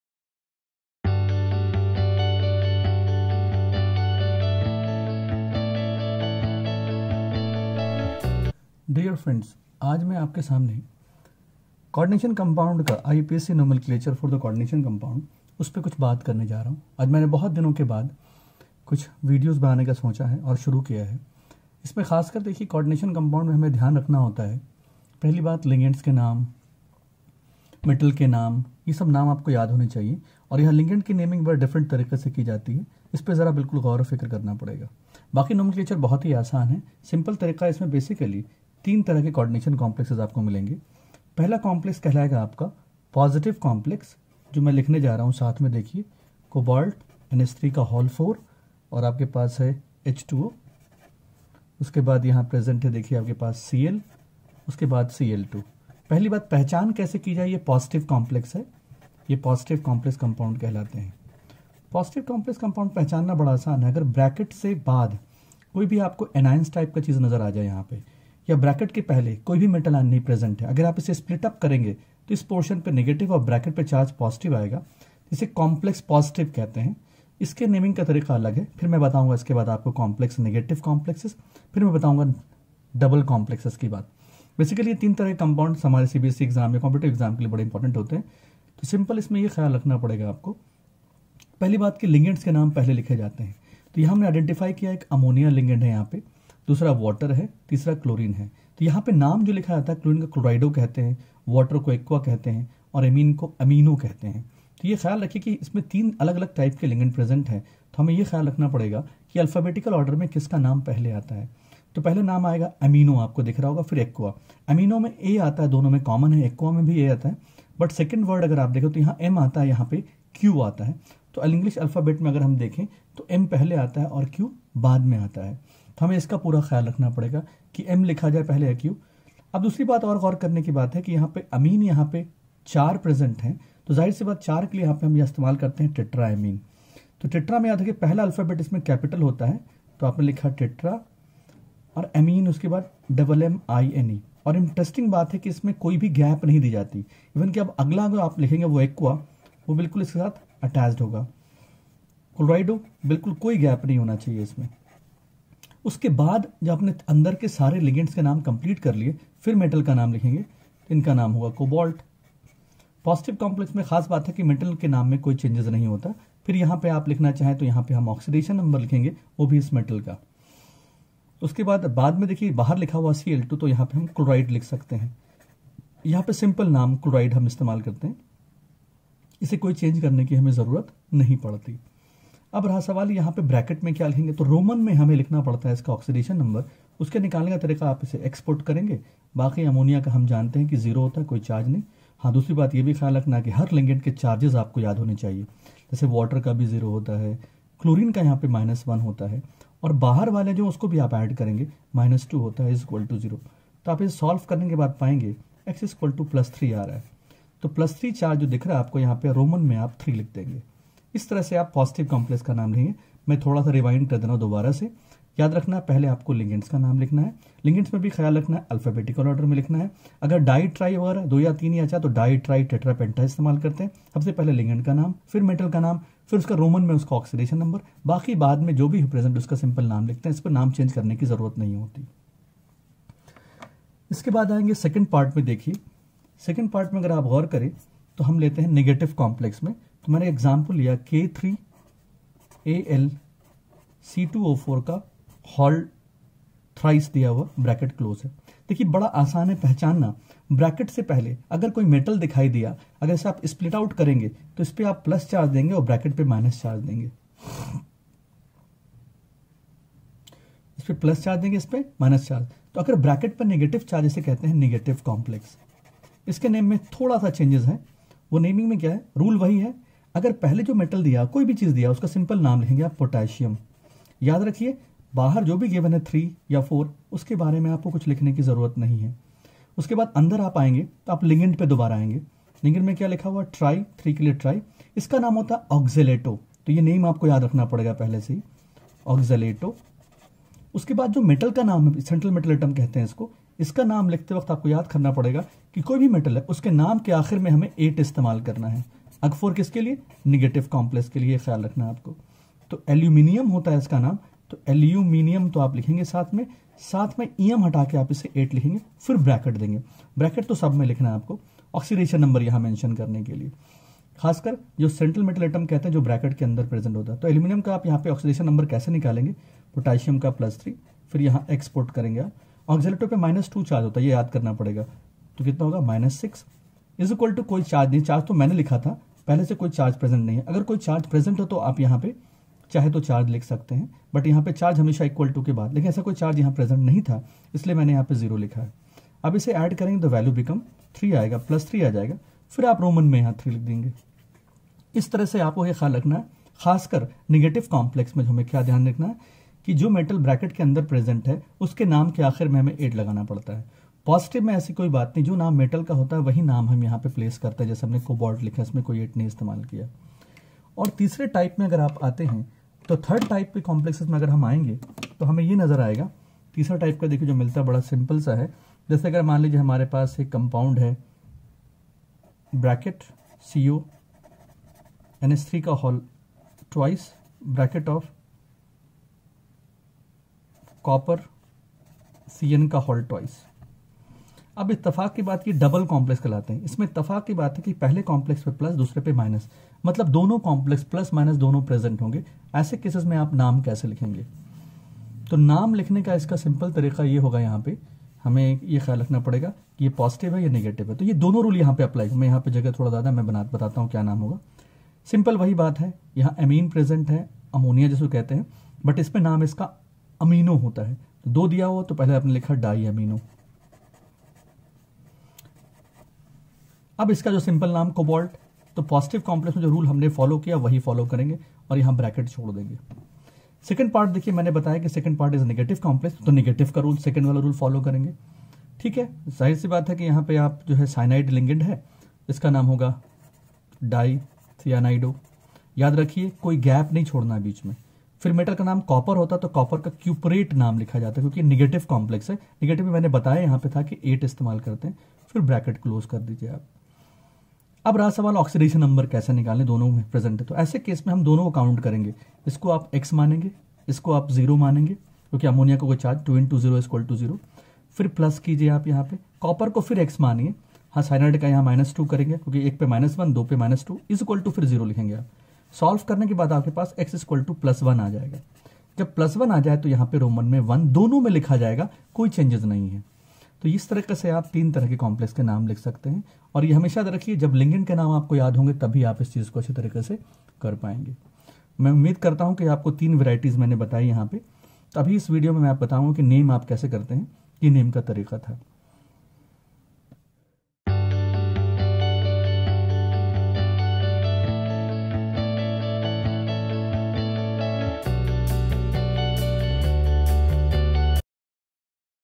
02-IUPAC Nomenclature|Coordination Compounds|[Hindi/Urdu] Part-1|Dr. Ataur Rahman
Iupac nomenclature
Coordination Compounds
Class 12th chemistry basic concepts,
For IITJEE, NEET exam